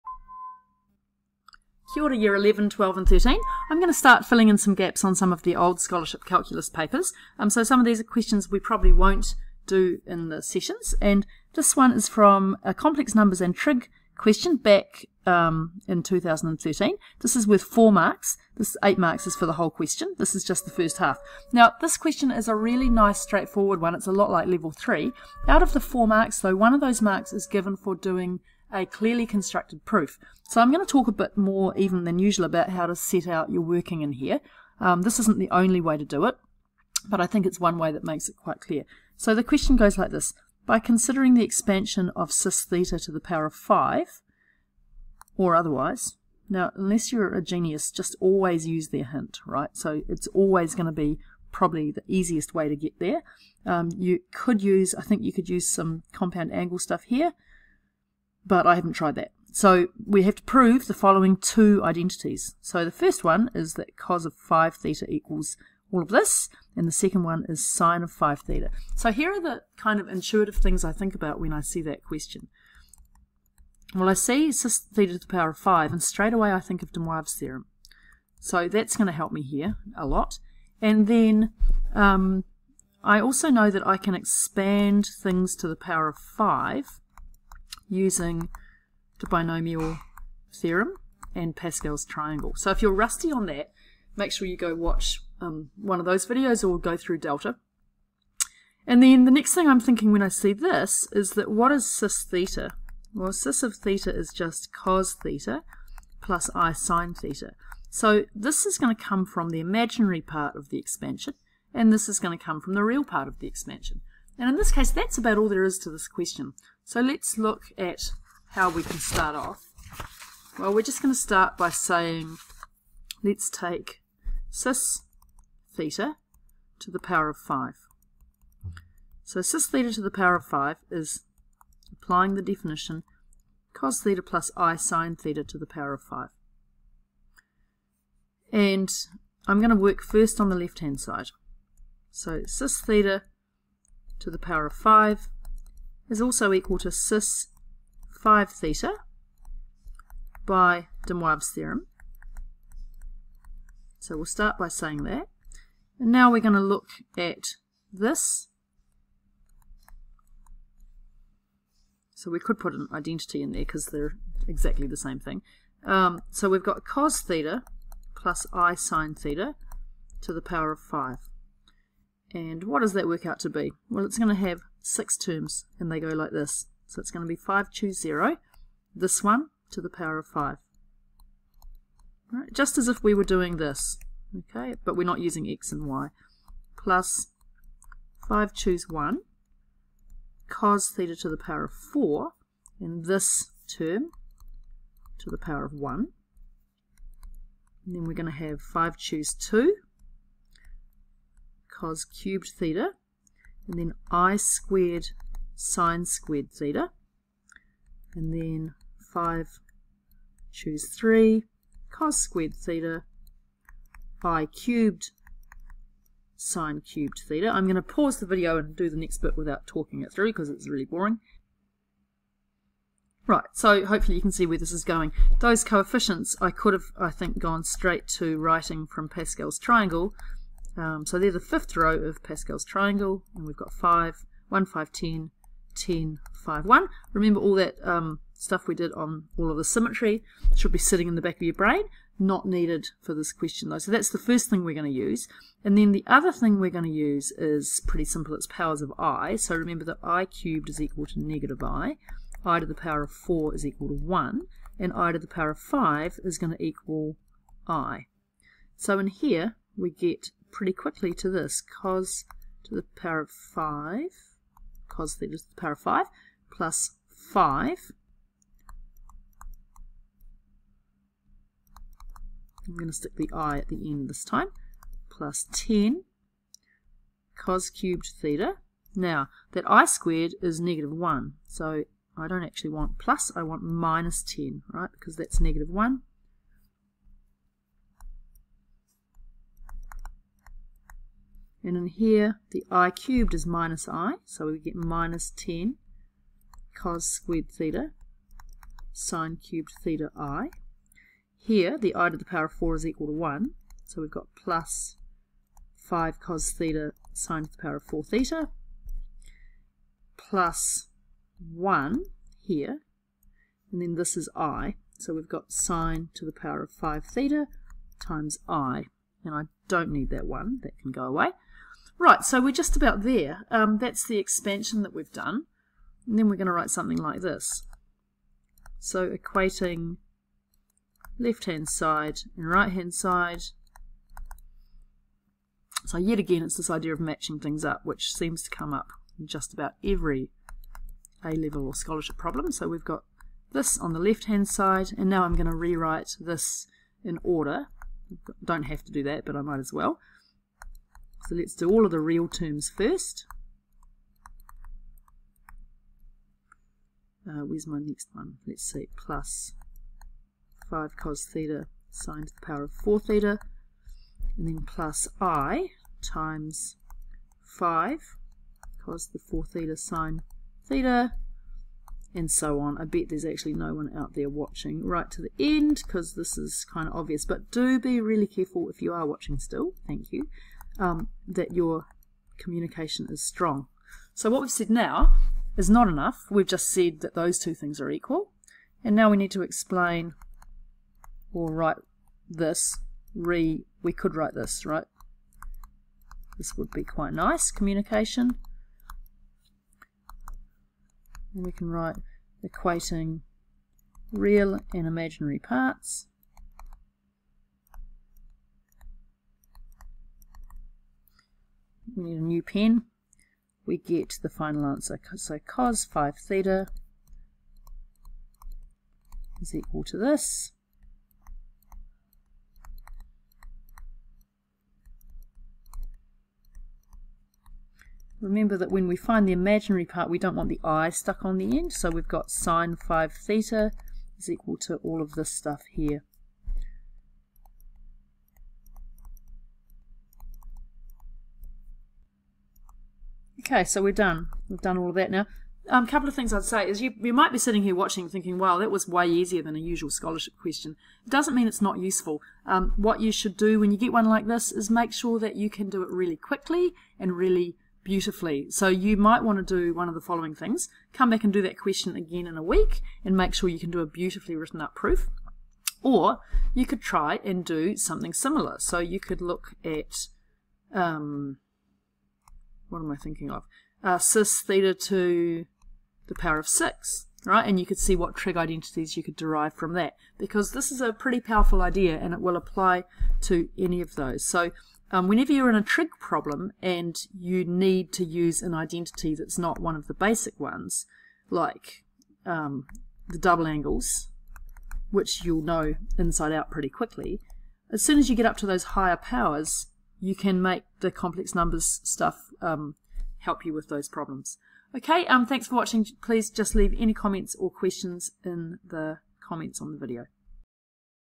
Scholarship Calculus 2013 Q6(a) Easy complex/trig problem
Proving identities for cos(5theta) and sin(5theta). Use this question to make sure you are communicating your thinking clearly.